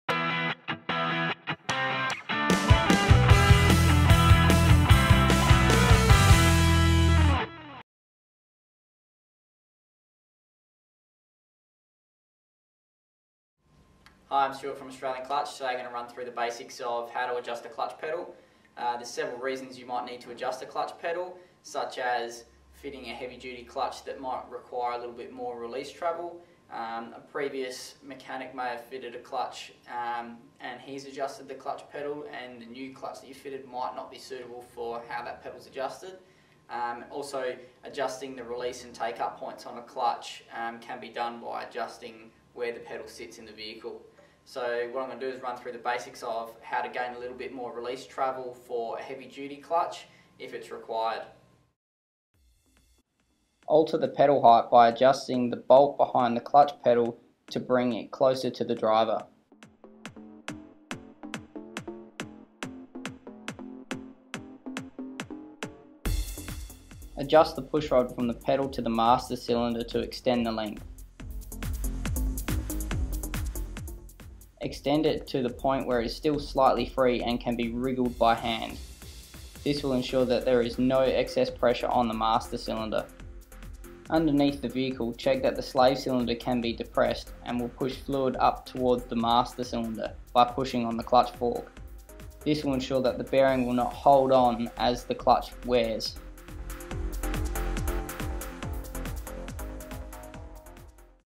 CLUTCH TECH: Hydraulic Clutch Pedal Adjustment
A properly adjusted clutch pedal can improve the feel of the clutch. On Clutch Tech, we take you through the basics of adjusting your hydraulic clutch pedal.